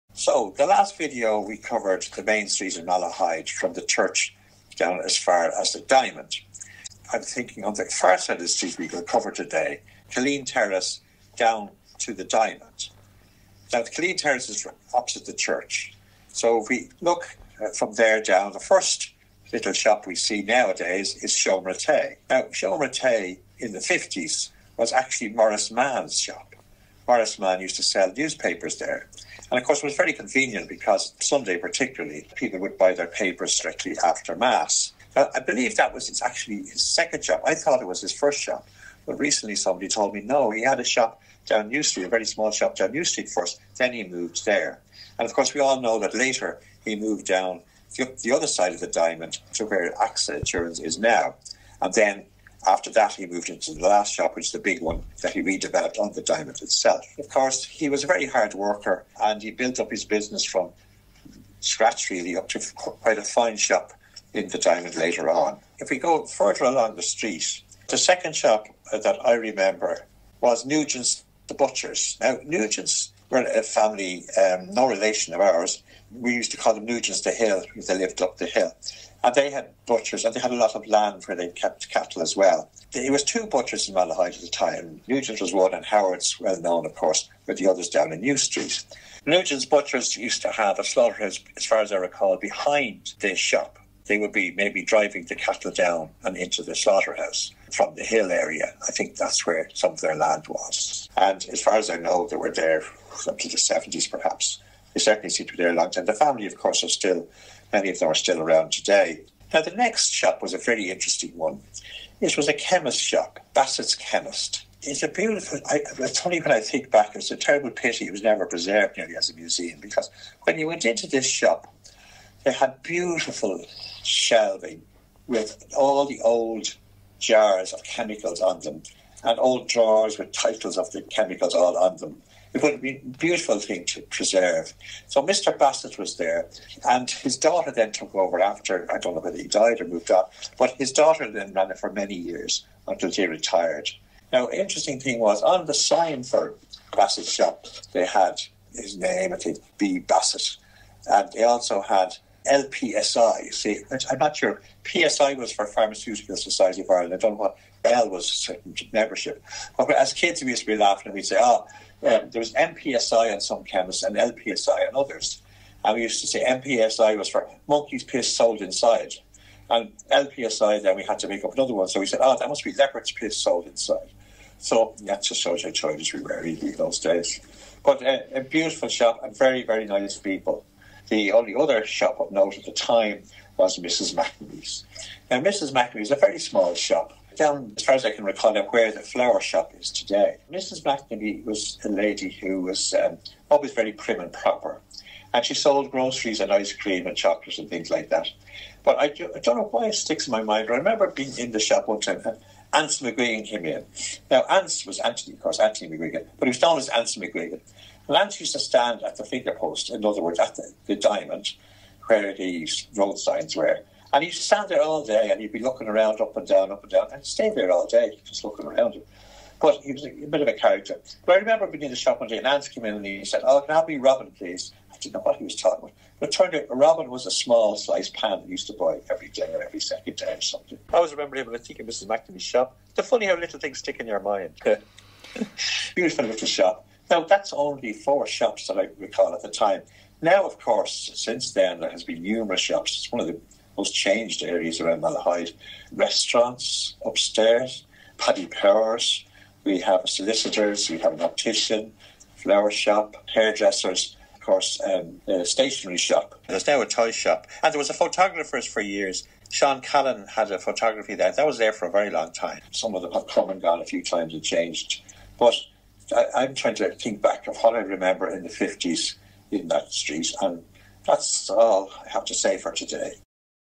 Malahide Memories 8: Main Street P2
Eddie Nugent talks about Main Street from Killeen Terrace down to the diamond including the original Maurice Mahon's, Nugent's butchers, Bassett's pharmacy and Mrs McNamee's shop.